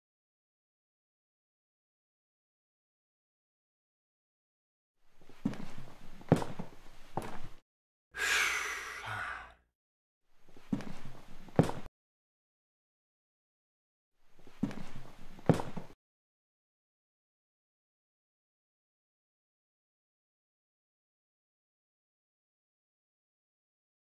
GS Test 1
Teaching myself how to work with green screen footage. This is a still of my DVD collection combined with an actor shot in front of a green screen and added foley (footsteps).